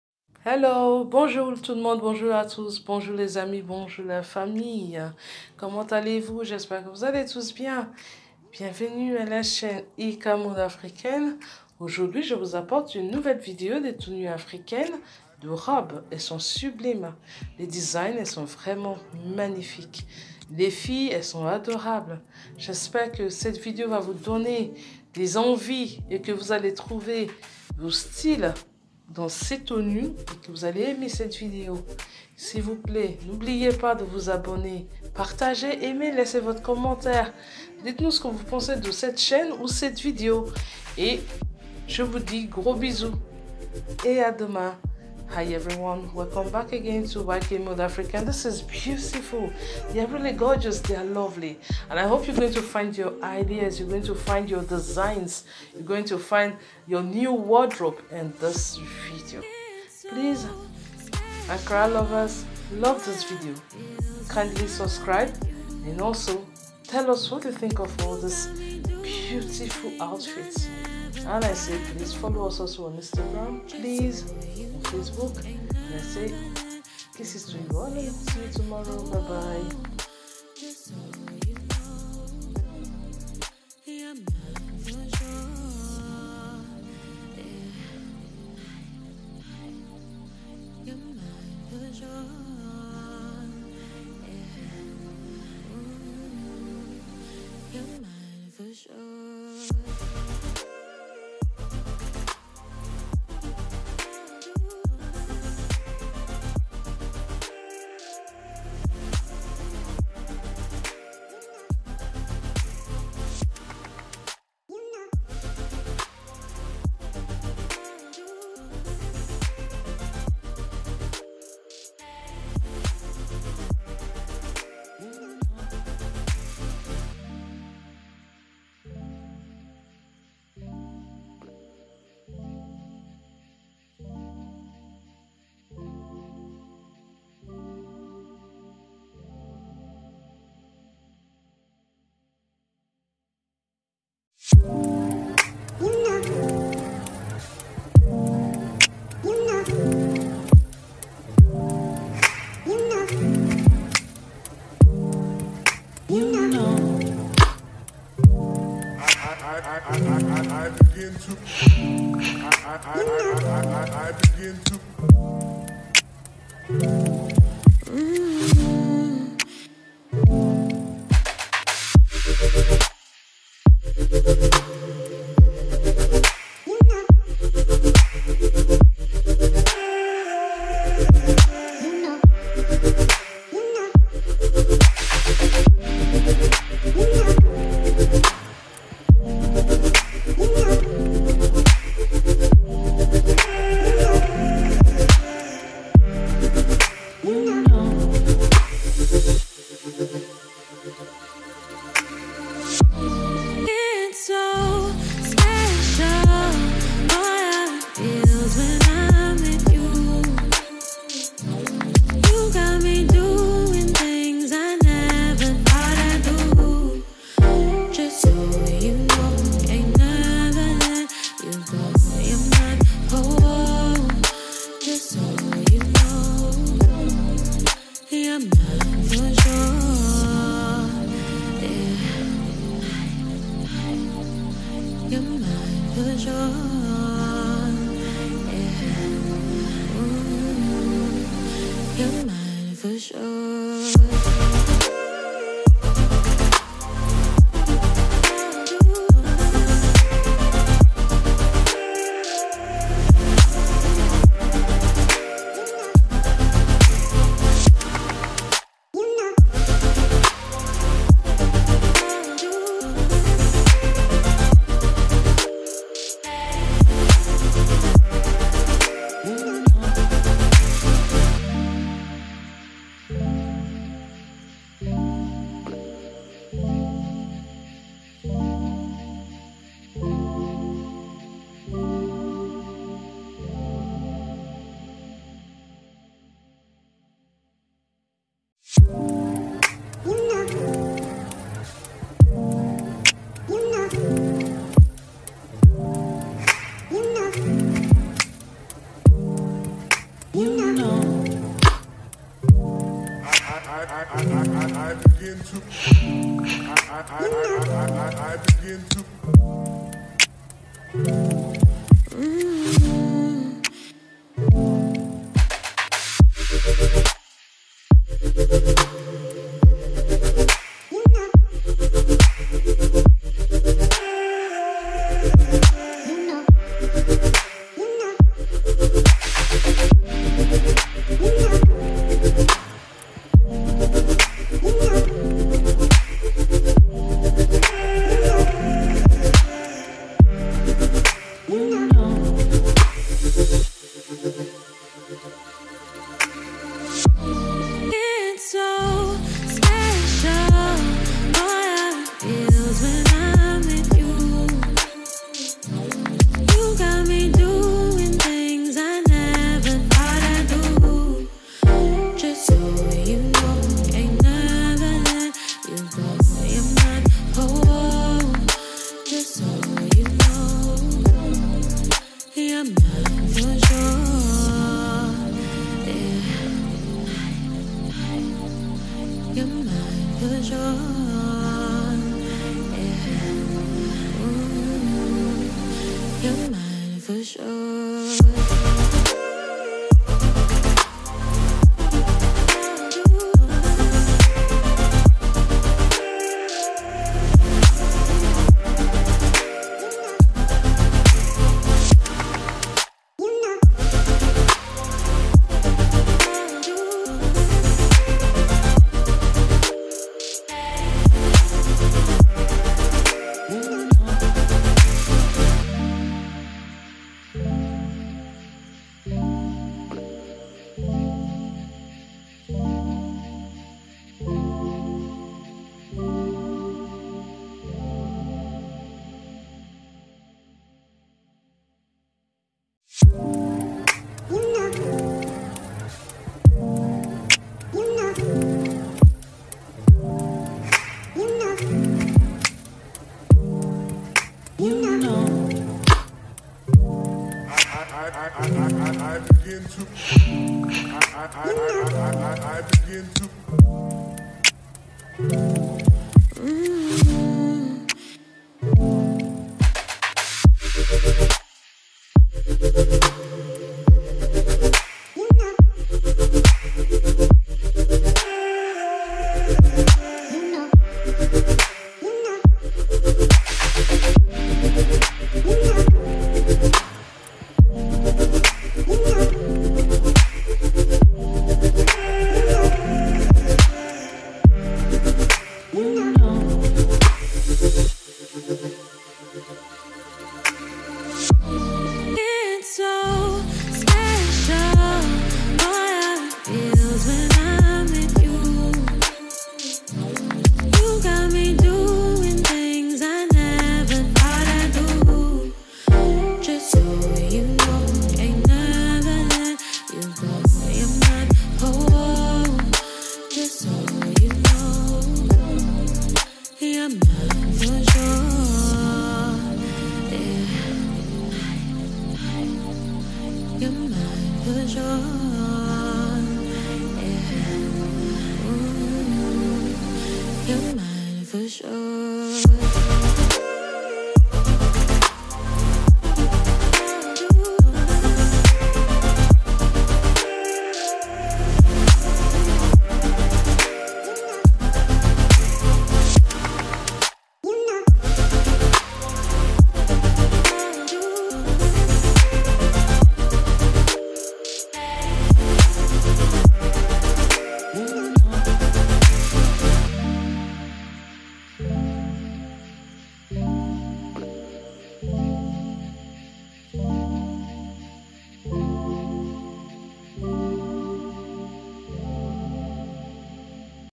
🆕️TRÈS BELLE TENUE AFRICAINE DE ROBE LONGUE MOYENNE 2023 WAX/PAGNE AFRICAN CASUAL DRESS ANKARA GOWN
Aujourd'hui c'est une nouvelle vidéo que je vous apporte,
vidéo de robe longue boubou très jolie j'espère que cette vidéo va vous donner des excellente idée.

vous pouvez passer votre commande, il suffit de faire capture d'écran, envoyer le modèle sur WhatsApp et nous vous donnons le prix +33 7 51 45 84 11